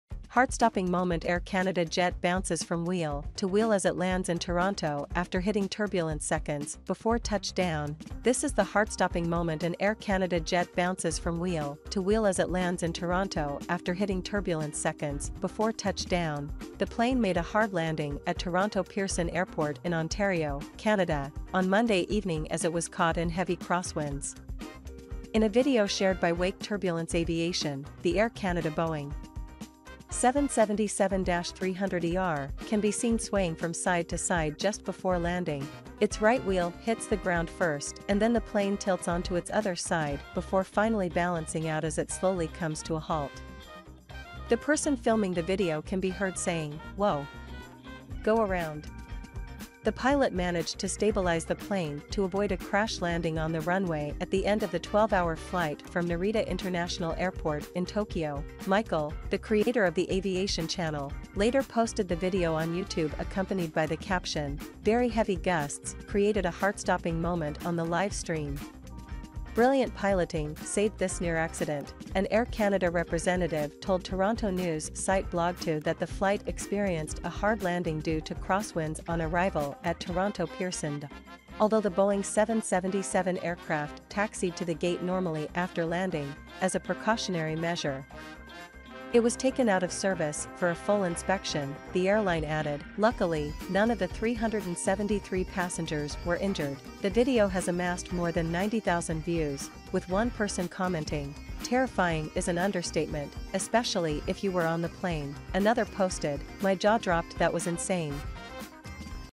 Air Canada jet bounces from wheel to wheel as it lands in Toronto
Heart-stopping moment Air Canada jet bounces from wheel to wheel as it lands in Toronto after hitting turbulence seconds before touch down
This is the heart-stopping moment an Air Canada jet bounces from wheel to wheel as it lands in Toronto after hitting turbulence seconds before touch down.
The plane made a hard landing at Toronto Pearson Airport in Ontario, Canada, on Monday evening as it was caught in heavy crosswinds.
In a video shared by Wake Turbulence Aviation, the Air Canada Boeing 777-300ER can be seen swaying from side to side just before landing.
Its right wheel hits the ground first and then the plane tilts onto its other side before finally balancing out as it slowly comes to a halt.
The person filming the video can be heard saying: 'Whoa! Go around'.
The pilot managed to stabilize the plane to avoid a crash landing on the runway at the end of the 12-hour flight from Narita International Airport in Tokyo.
Michael, the creator of the aviation channel, later posted the video on YouTube accompanied by the caption: 'Very heavy gusts created a heart-stopping moment on the live stream! Brilliant piloting saved this near-accident!'
An Air Canada representative told Toronto news site blogTo that the flight 'experienced a hard landing due to crosswinds on arrival at Toronto Pearson'.
'Although the Boeing 777 aircraft taxied to the gate normally after landing, as a precautionary measure, it was taken out of service for a full inspection,' the airline added.
Luckily, none of the 373 passengers were injured.
The video has amassed more than 90,000 views, with one person commenting: 'Terrifying is an understatement, especially if you were on the plane.'
Another posted: 'My jaw-dropped that was insane!!'